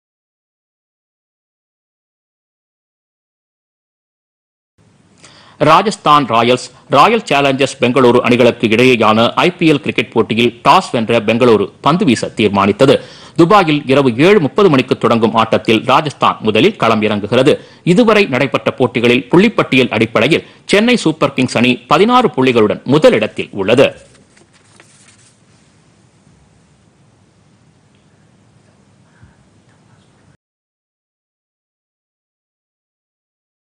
ஐபிஎல் கிரிக்கெட்-ராஜஸ்தான்-பெங்களுரு மோதல்
ஐபிஎல் கிரிக்கெட் - ராஜஸ்தான்  -  பெங்களூரு  அணிகள் இன்று மோதல்.